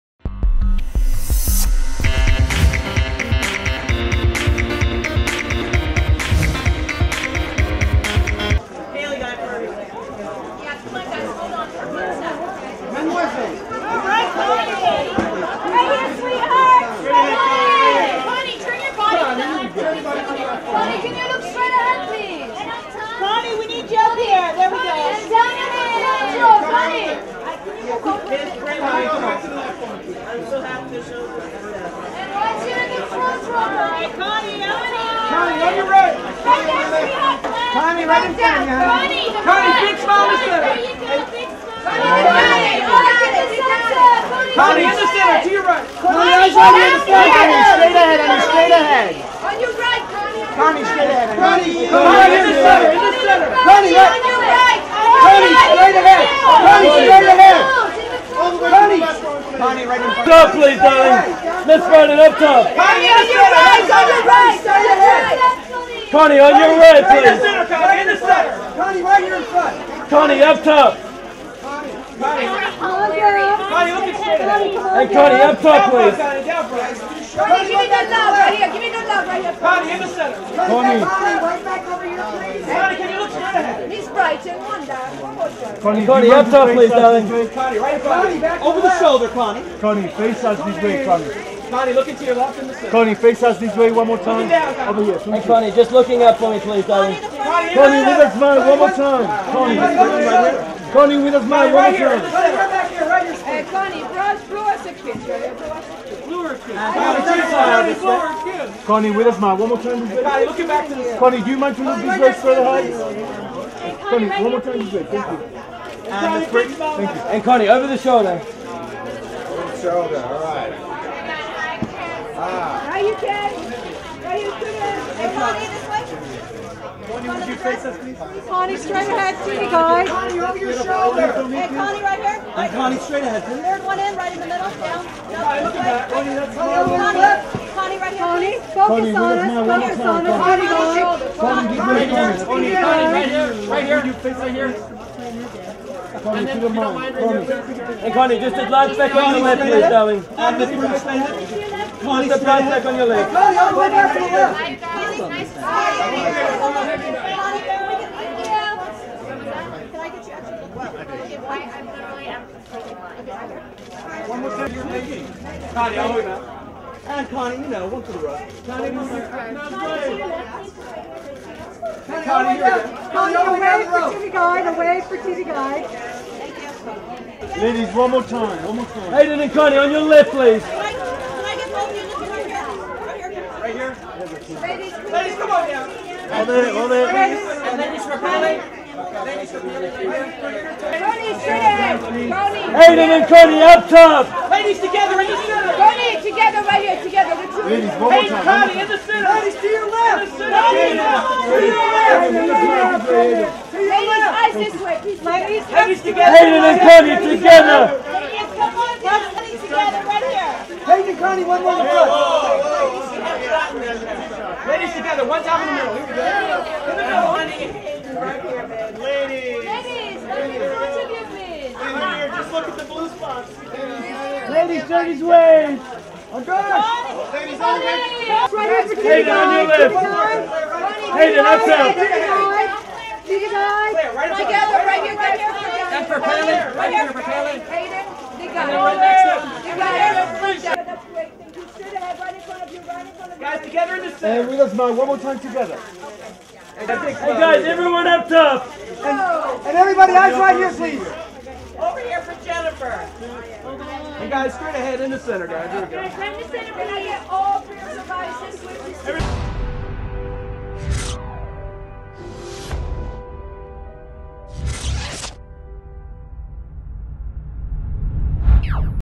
Connie Britton so radiant in a blue dress arriving at Nashville Panel PaleyFest 2013
Connie Britton arrives at "Nashville" Panel during The 30th Annual PaleyFest: The William S. Paley Television Festival March 09, 2013 - Saban Theatre - Beverly Hills, California, United States